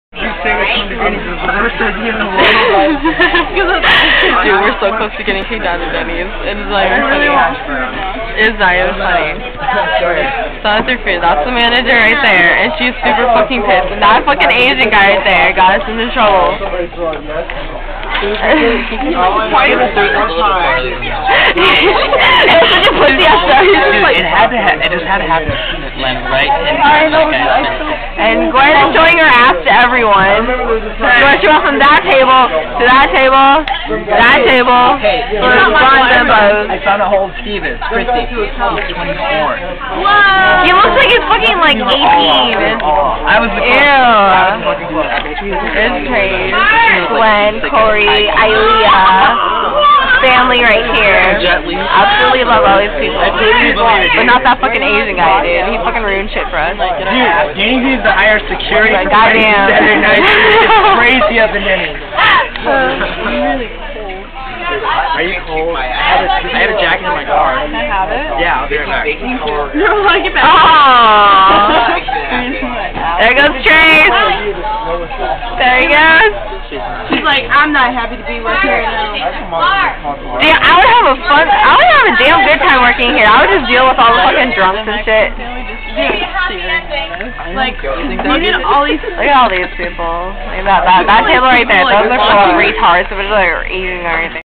An epic night at Dennys
Hahahah this has been my best experience at Dennys so far!

Drunk people are hilarious